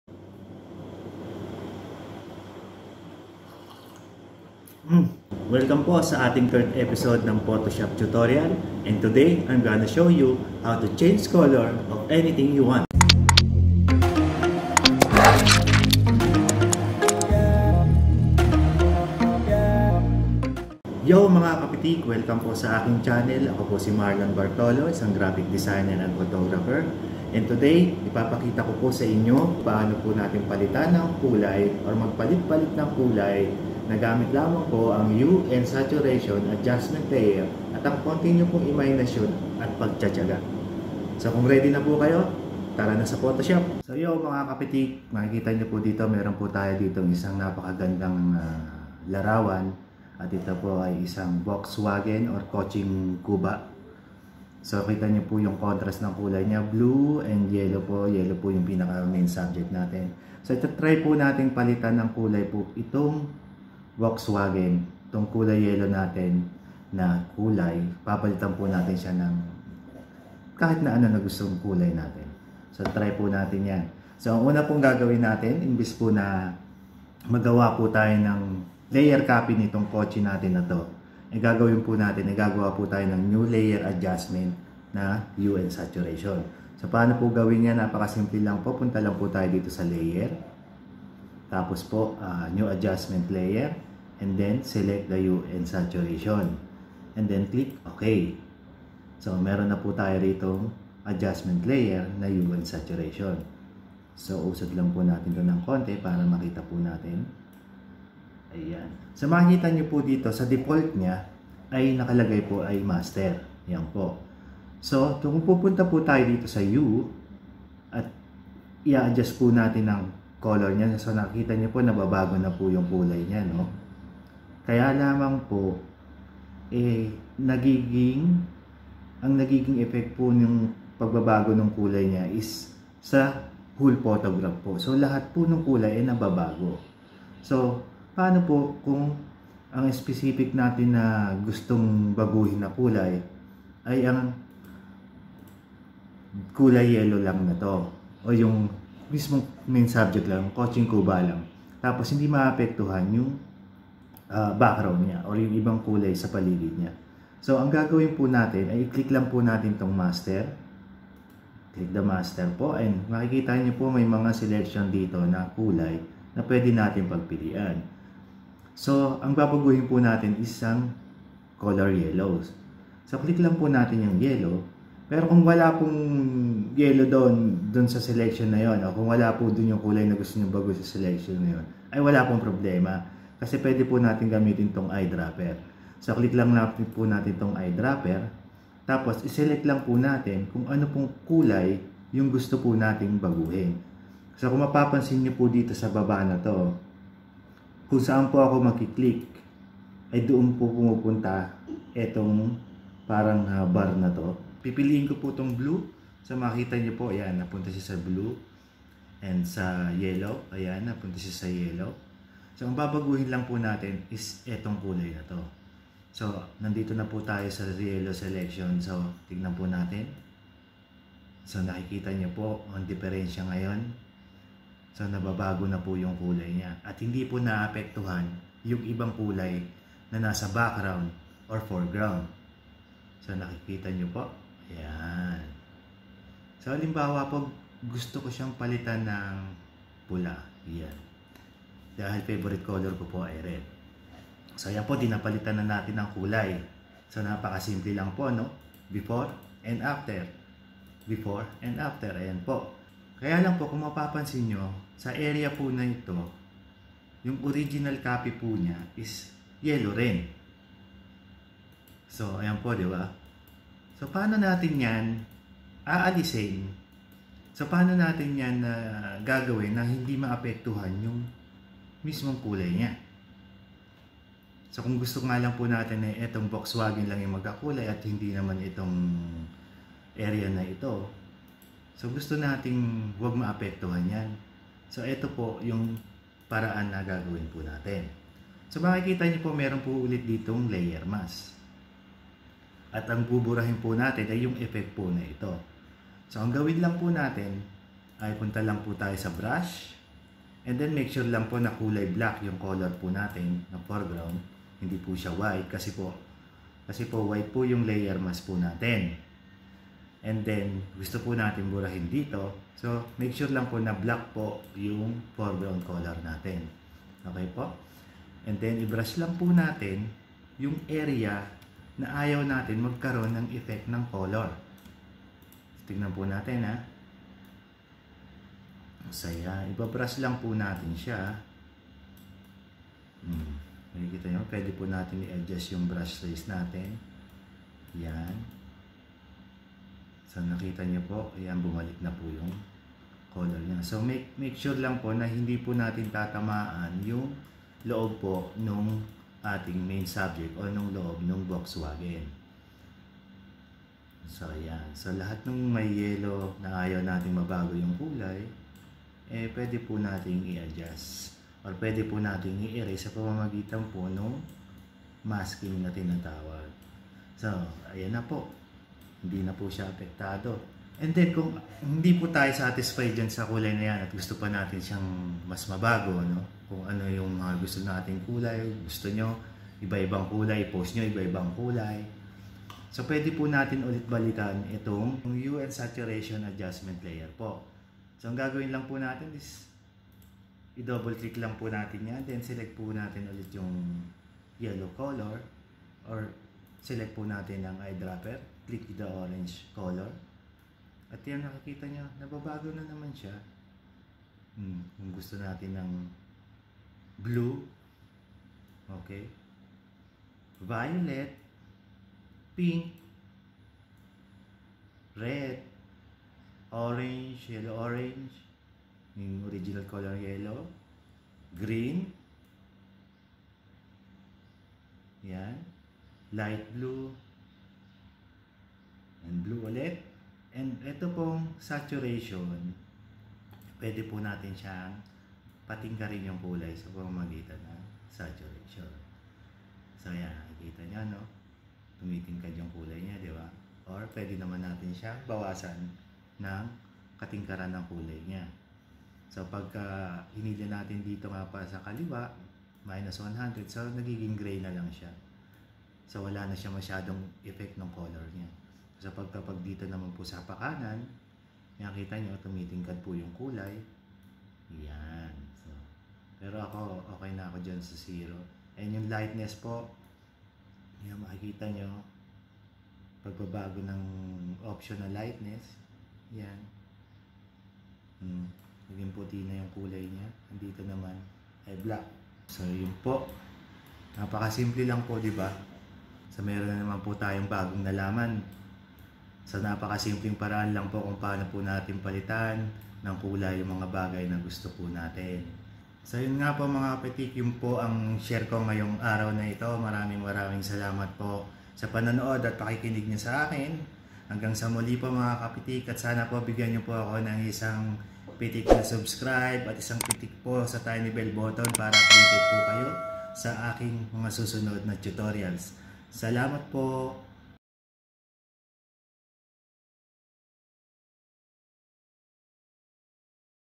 How to Change Color in Photoshop Ep.03 (Tagalog Tutorial )
Yo! Ka-Pitik! welcome po sa ating 3rd episode ng photoshop tutorial! and today I'm gonna show you how to change color of anything you want!

Salamat po ng Marami :) God Bless to all !

                        : MB_Artfood